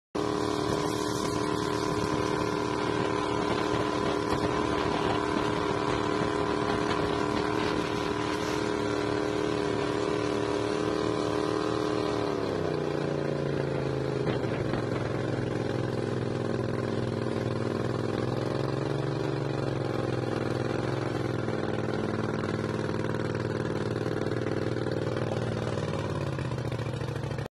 23hp surface drive, gatortail doing 31.6 mph
stage 3 23hp vanguard . ratio rods, arai pistons, manley valves, .308 cam, heads ported,  modified 34mm mikuni , black death intake, and Robertson torque tube exhaust.  1.39 gear ratio . ca57 prop